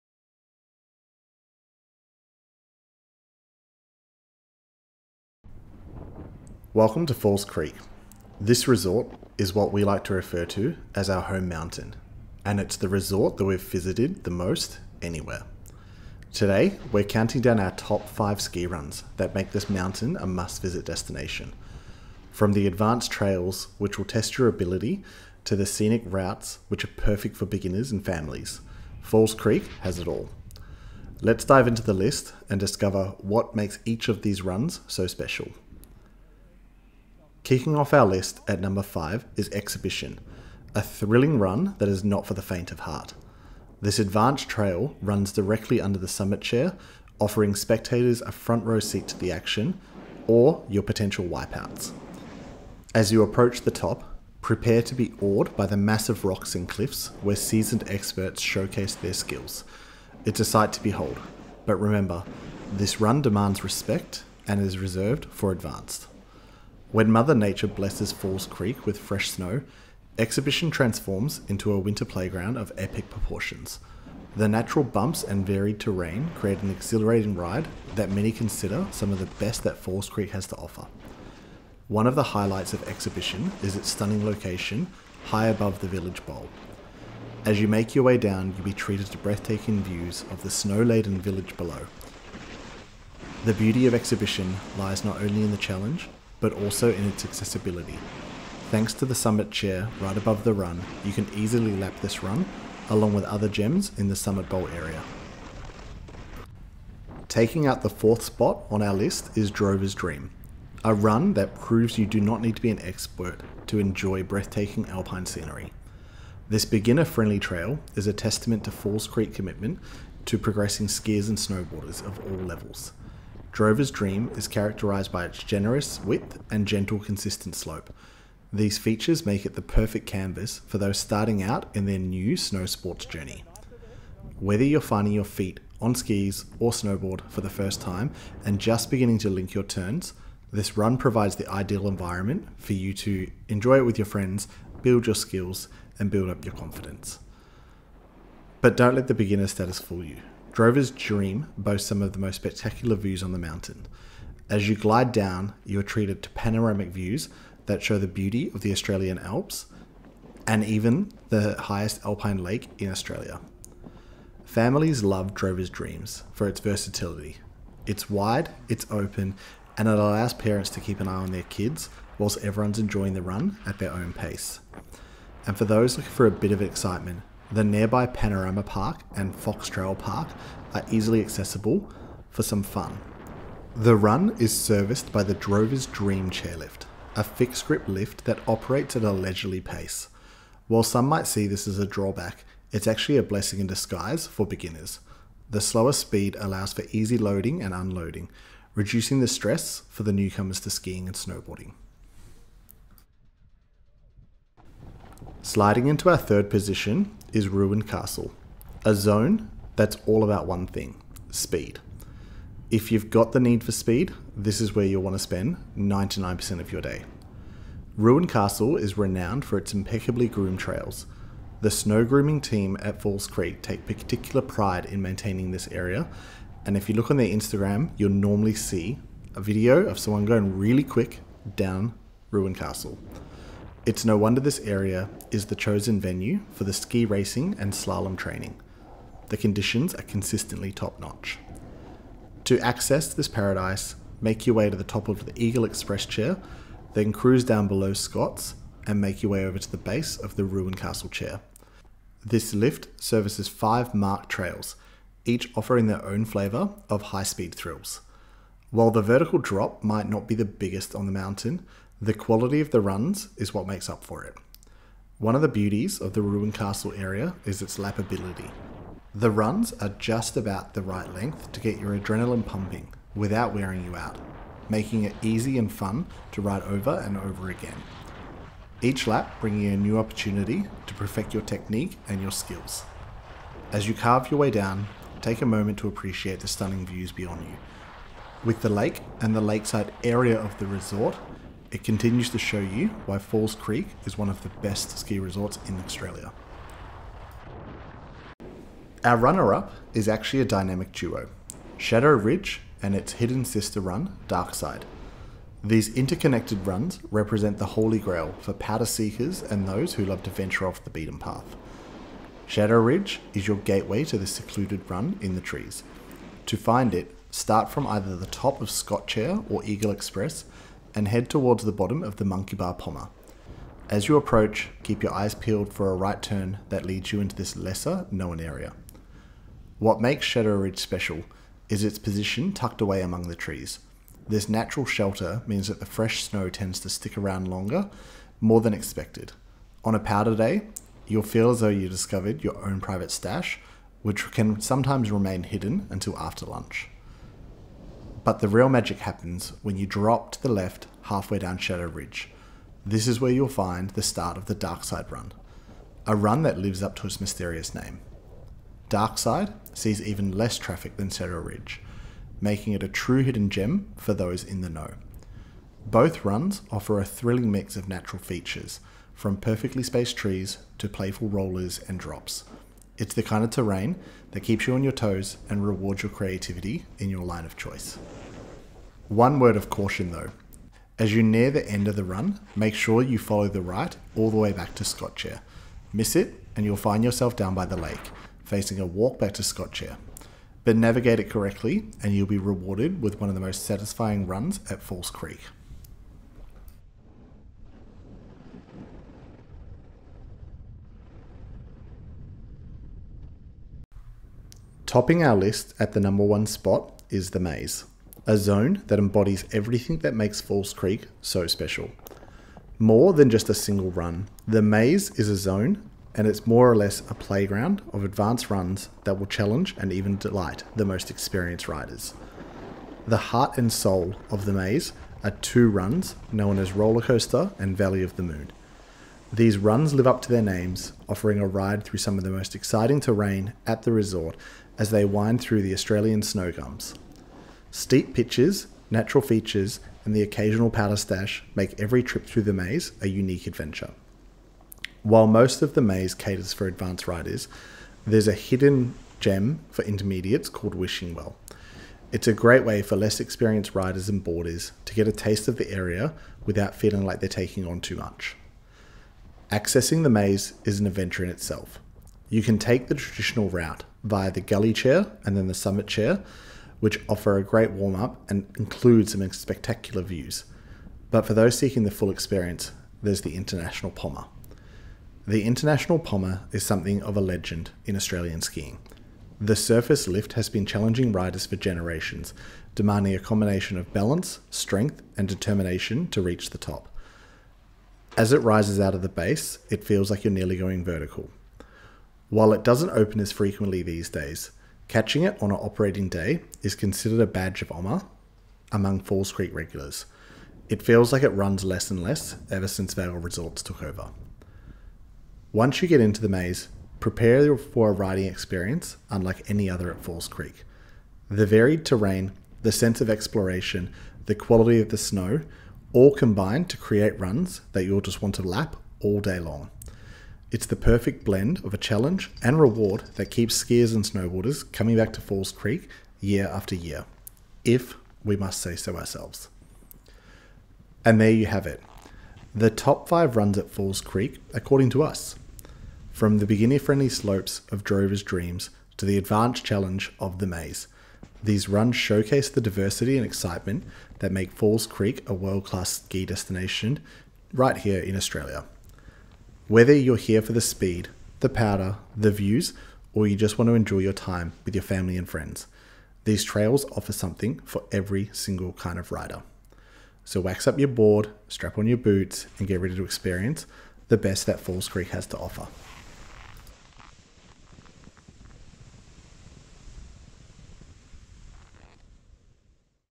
Falls Creek Unveiled: Top 5 Ski Runs You Can't Miss!
Discover the top 5 ski runs at Falls Creek, Australia's premier alpine resort! From the adrenaline-pumping Exhibition to the scenic Drovers Dream, we'll guide you through the best slopes this mountain has to offer. Whether you're a speed demon, powder hound, or just starting out, there's something for every skill level. Join us as we explore the thrilling Ruined Castle, the hidden gems of Shadow Ridge and Darkside, and the legendary Maze. Get ready for breathtaking views, expert tips, and insider knowledge to make your next Falls Creek adventure unforgettable!